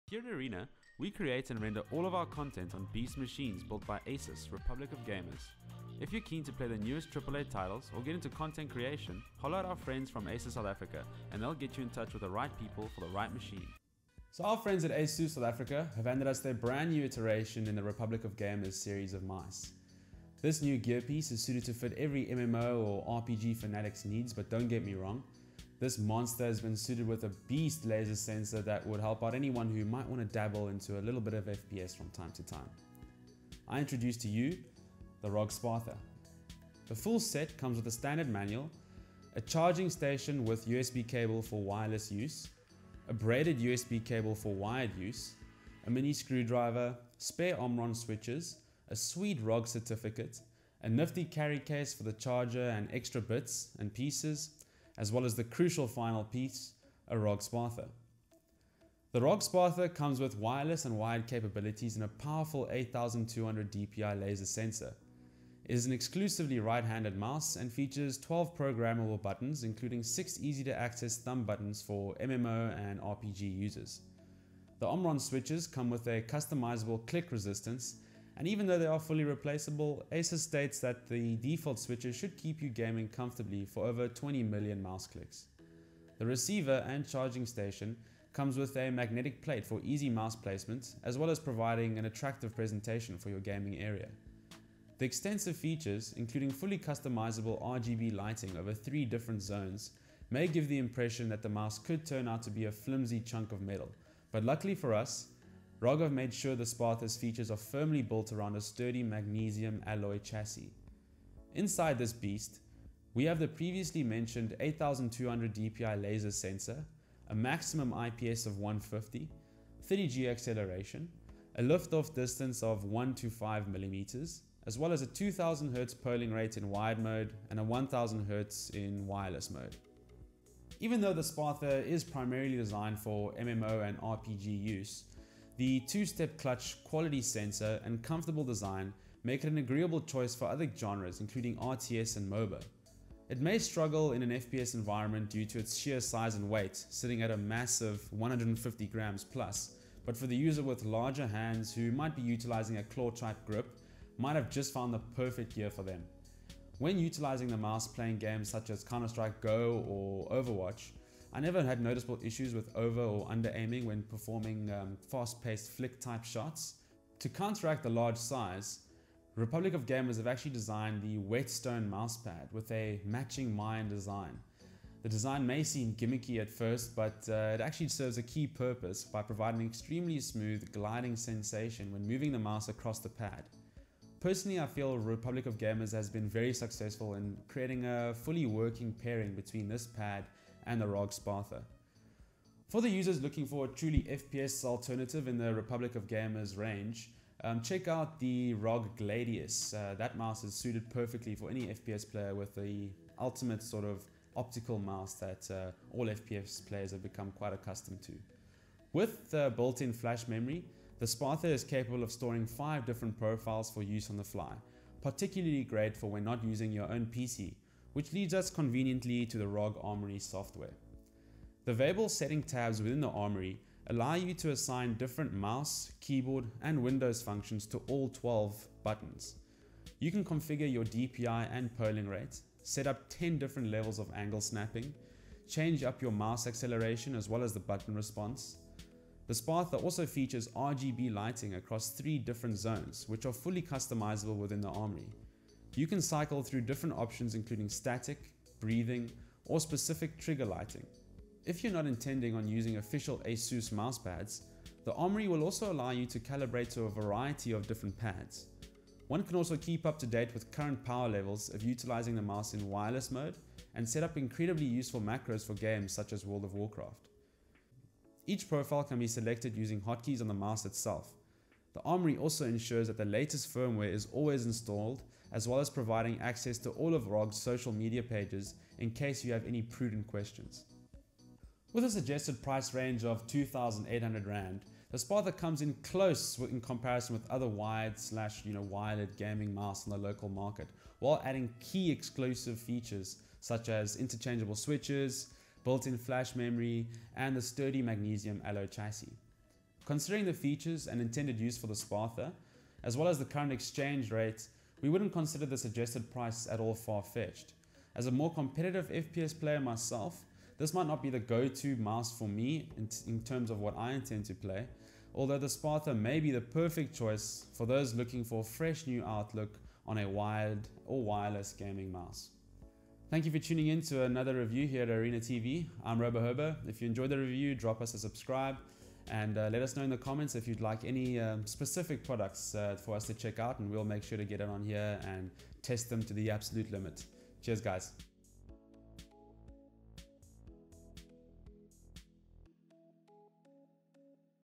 ASUS ROG Spatha Review - Could this be the best yet?
Luca "RoBoHoBo" Tucconi from Orena reviews ASUS' very own wired/wireless gaming mouse, the ROG SPATHA!

All content produced on machines built by ASUS South Africa!

Follow OrenaTV for more!